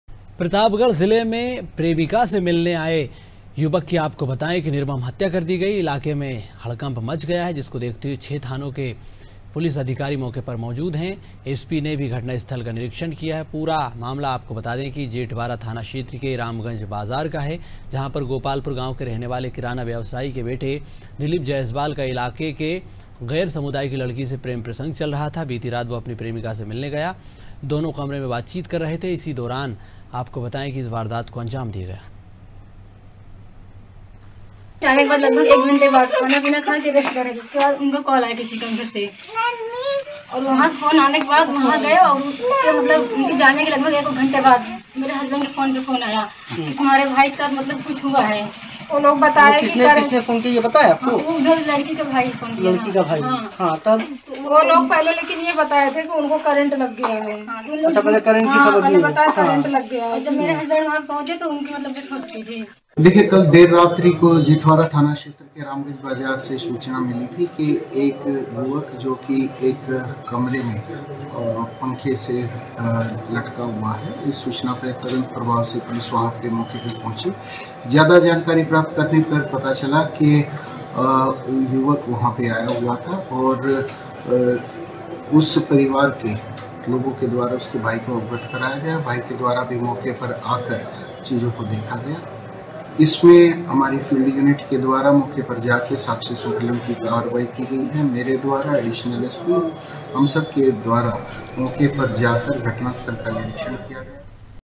Pratapgarh : प्रेमिका से मिलने गए प्रेमी की हत्या, फंदे से लटकता मिला युवक का शव || Anaadi Tv
प्रतापगढ़ जिले में गैर समुदाय की प्रेमिका से मुलाकात करने आधी रात उसके घर पहुंचे प्रेमी की निर्मम हत्या से हड़कम्प मचा हुआ है...इलाके में जबरदस्त तनाव है...जिसे देखते हुए 6 थानों के पुलिस बल को तैनात किया गया है...वहीं एसपी ने भी घटनास्थल पहुंचकर मुआयना किया...पूरा मामला जेठवारा थाना क्षेत्र के रामगंज बाज़ार का है...जहां गोपालपुर गांव के रहने वाले किराना व्यवसायी के बेटा दिलीप जयसवाल का इलाके के गैर समुदाय की लड़की से प्रेम प्रसंग चल रहा था...बीती रात वो अपनी प्रेमिका से मिलने उसके घर पहुंच गया...दोनों कमरे में बातचीत कर रहे थे तभी लड़की के घरवालों को इसकी भनक लग गई और घर वालों ने प्रेमी के निर्मम हत्या करने के बाद...उसका शव फांसी के फंदे से लटका कर कमरा बंद कर फरार हो गए...

AIRTEL - 379